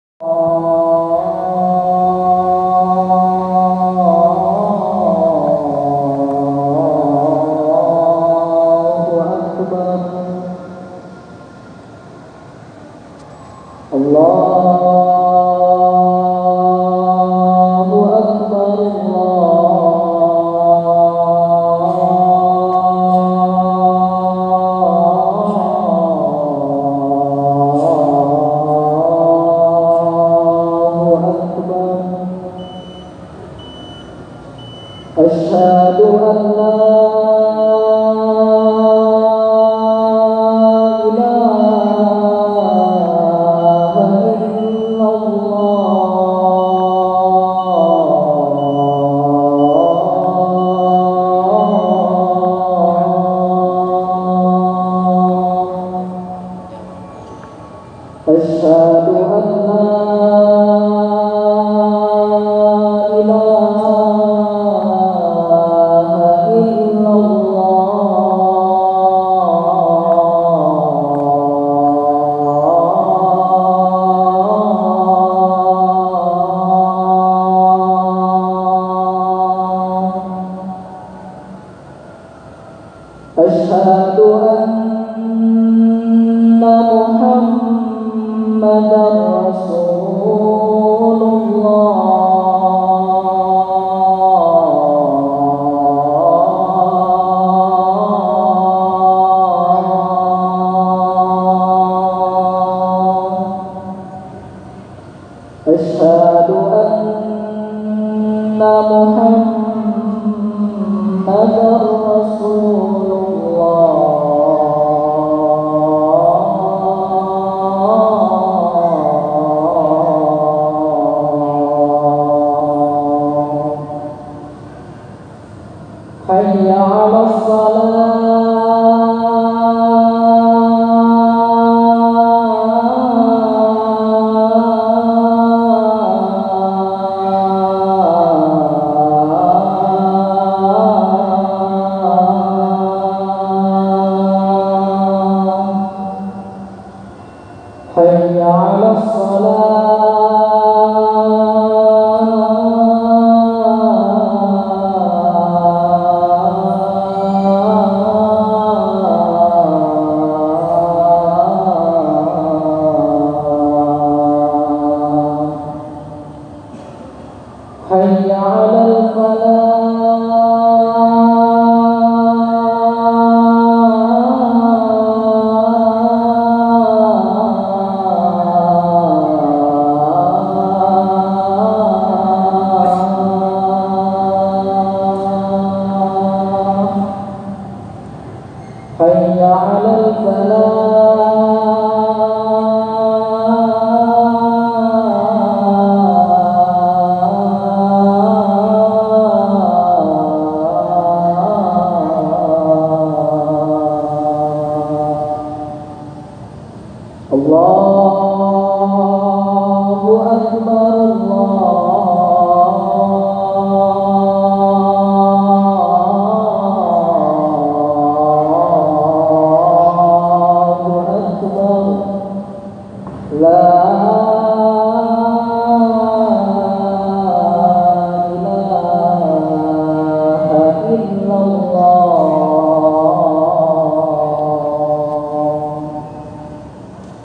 Adzan Isya di Masjid Al-Ihsan Kuala Kapuas
INFORMASI KAPUAS - Direkam pada hari Senin, 6 Mei 2019. Adzan dilantunkan oleh Abdul Latif.